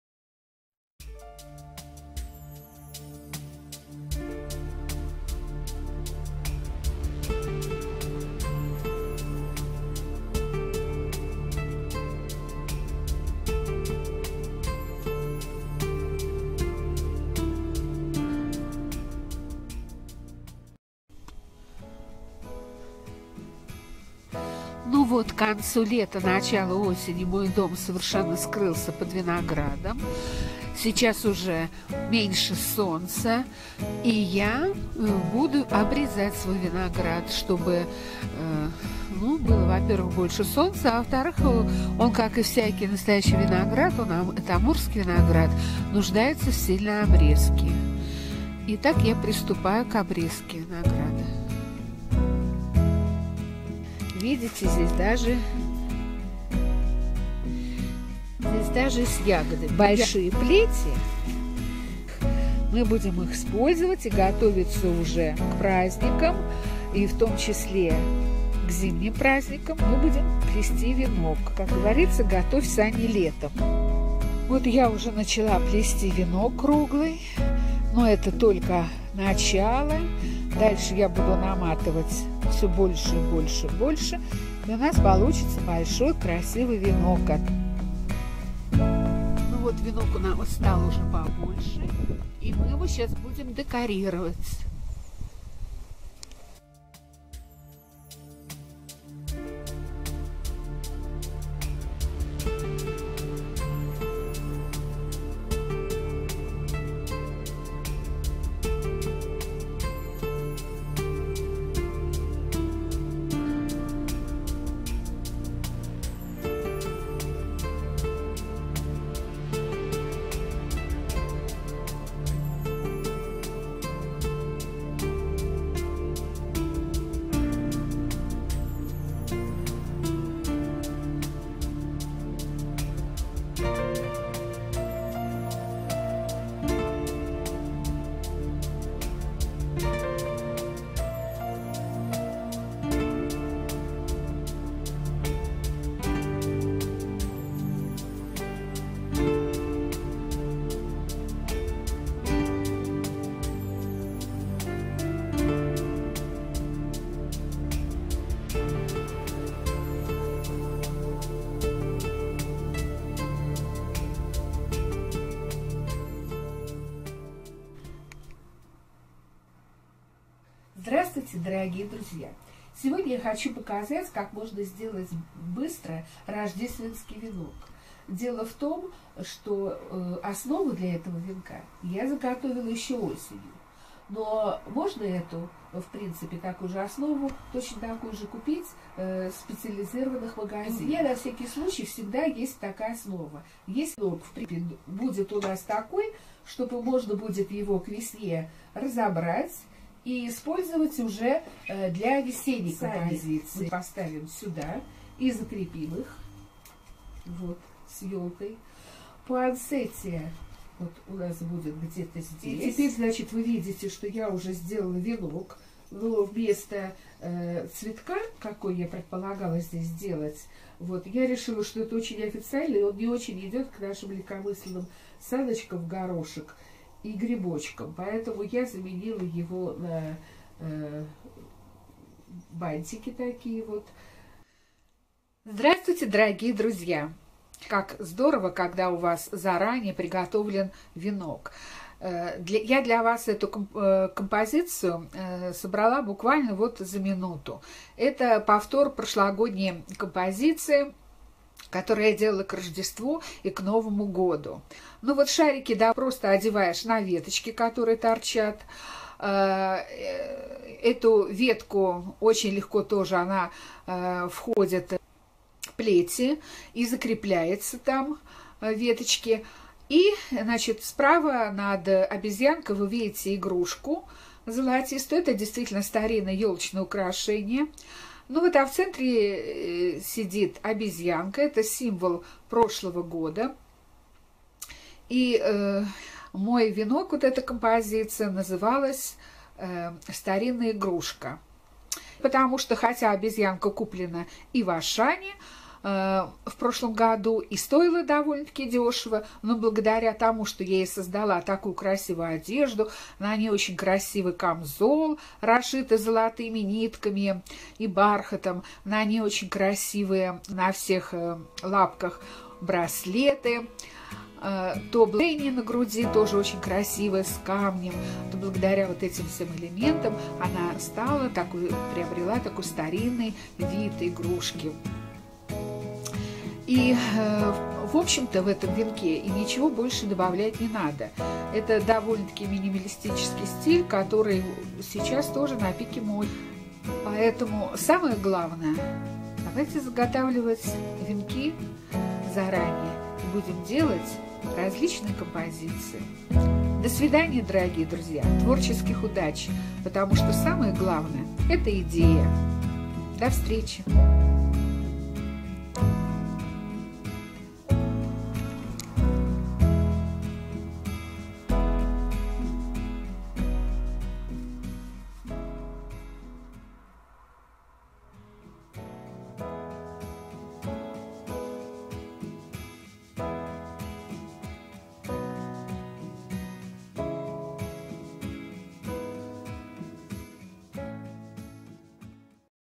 Новогодний декор. М.К.Рождественские венки из лозы - быстро, дешево и модно!!! Christmas wreaths.
Если есть заготовка венка - вы уже на 70% готовы к встрече Нового года! Плюс оригинальную идею - и ваш венок готов! Красивый и как говорят "бюджетный", экономный! Который прослужит несколько лет! Легко собирается и легко разбирается. Что может быть лучше!!! А если вы захотите его подарить - то это будет лучший подарок!!!